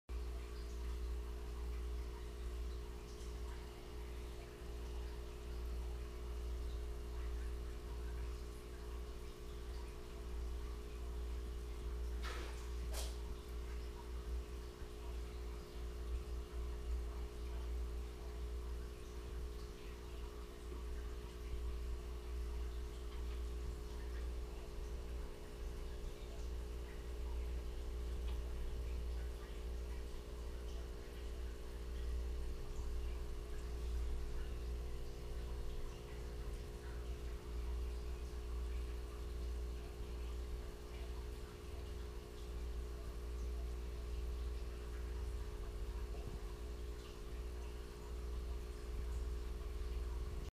Channa Gachua With Fry Part 4
Update on the fry.  I'm feeding them crushed NLS pellets.